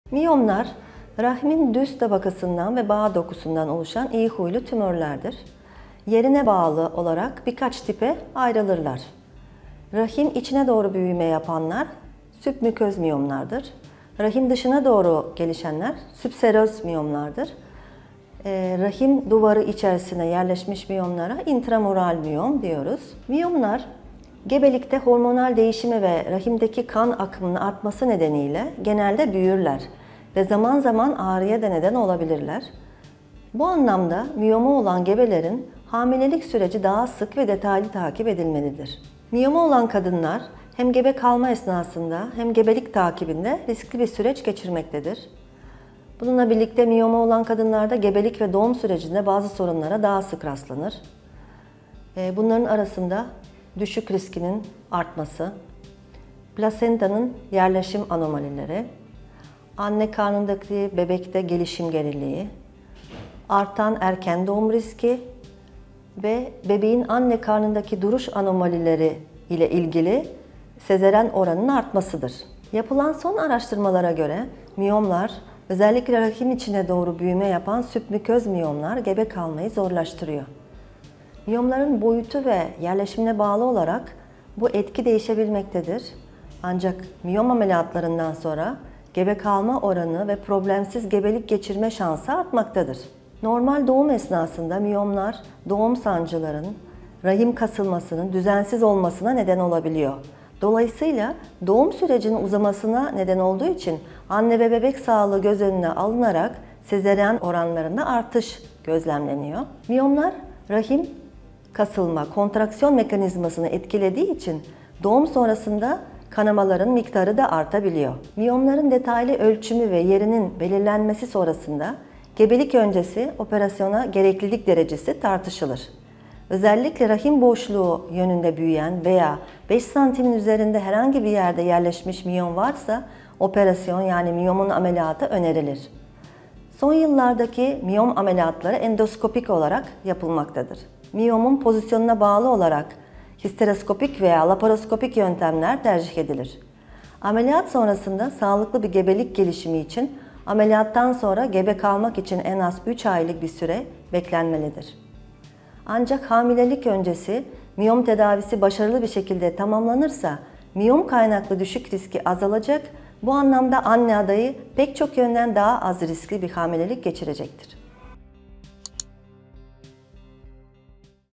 Miyom düşüğe sebep olur mu? | Uzman TV
Suzan Gençsoy Uzman TV için cevapladı: Miyom düşüğe sebep olur mu?